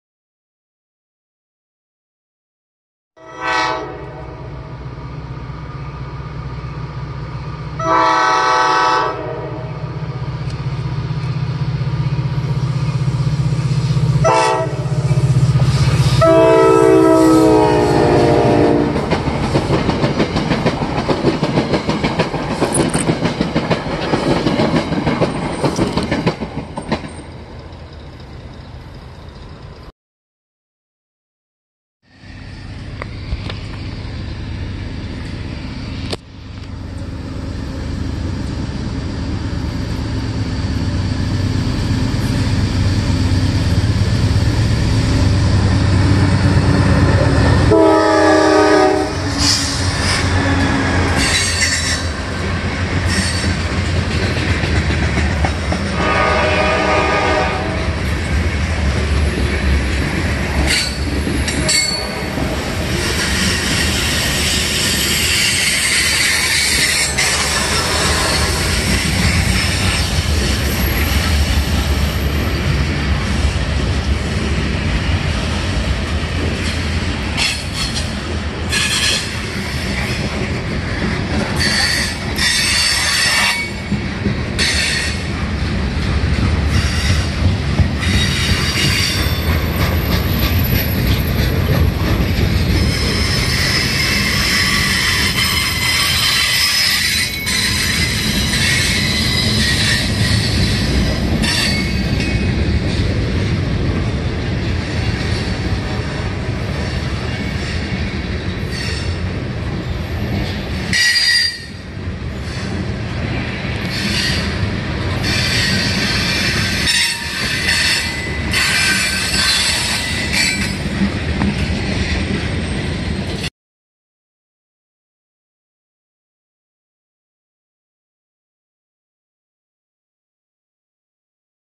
Norfolk Southern H70 at HO-HO-KUS, NJ
Norfolk Southern ran the H70 this Friday in the late morning. Caught it at HO-HO-KUS with seconds to spare.